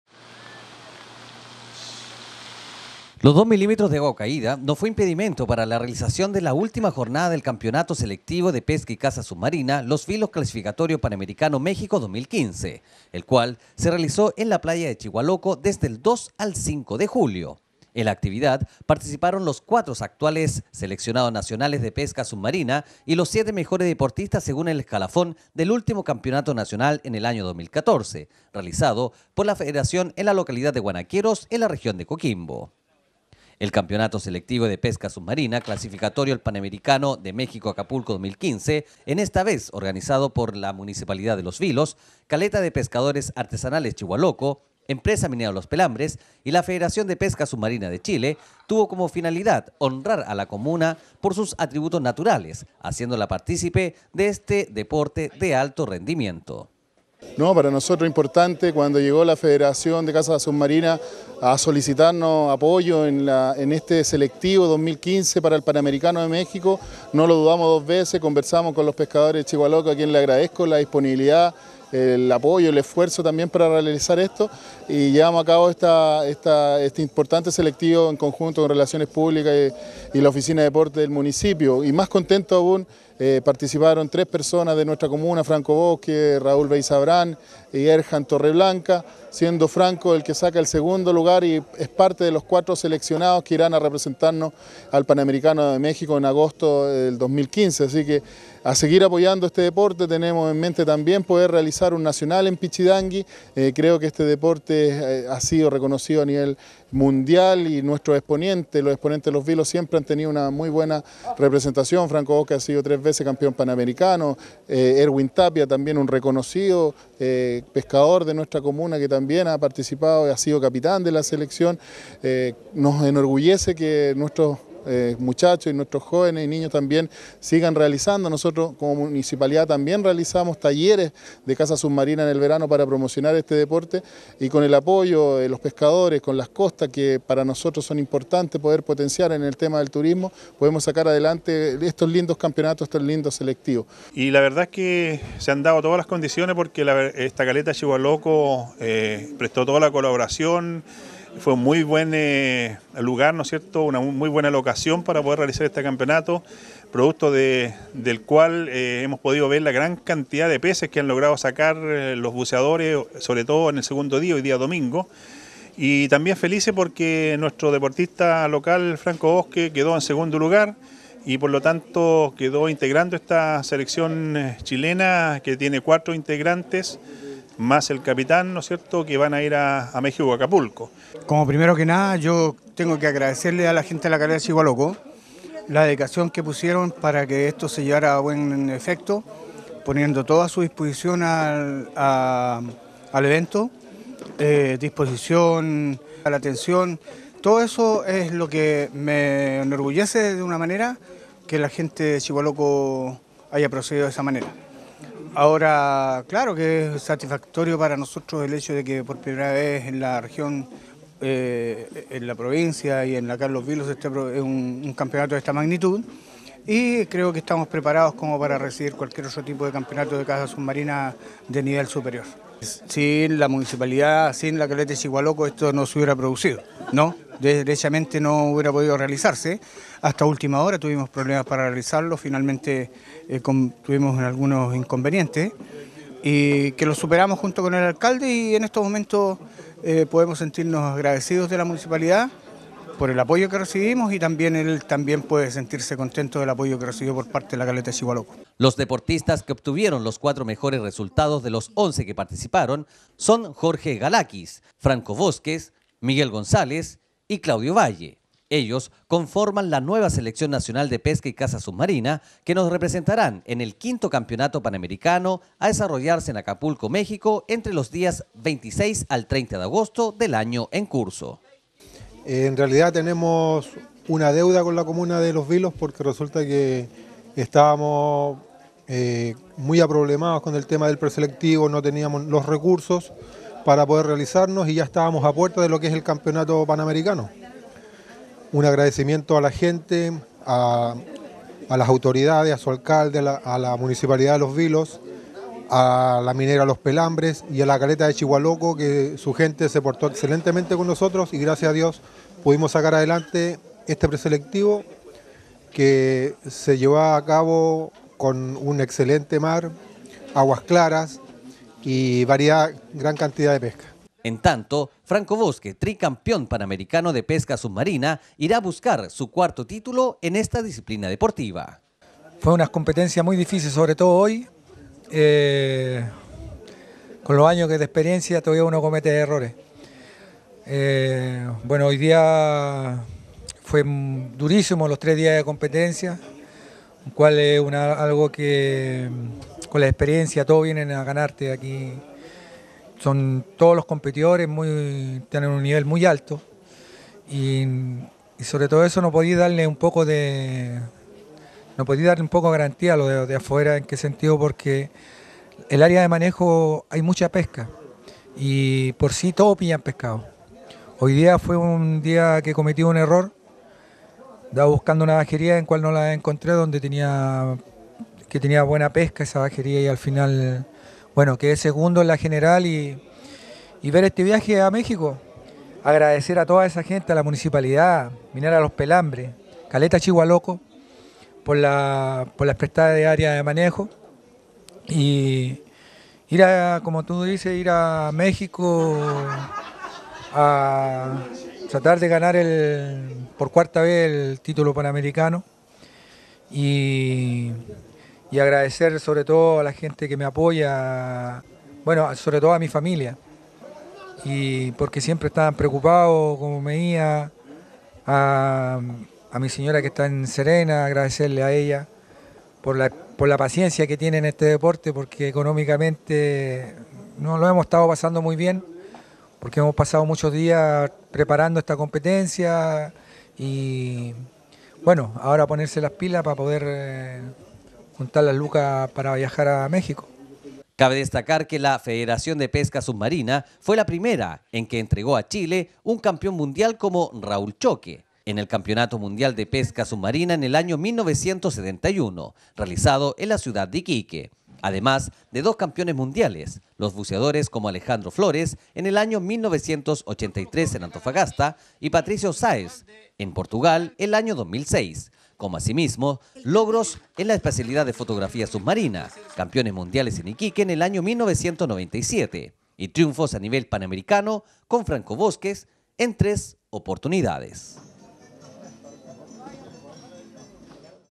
EN LA COMUNA DE LOS VILOS SE REALIZÓ LA CLASIFICATORIA DE PESCA SUBMARINA 2015.
El campeonato selectivo de pesca submarina, clasificatorio al panamericano de México Acapulco 2015, esta vez organizado por la Municipalidad de Los Vilos, Caleta de pescadores artesanales Chigualoco, Empresa MLP, y la Federación de pesca submarina de Chile, tuvo como finalidad honrar a la comuna por sus atributos naturales haciéndola participes de este deporte de alto rendimiento.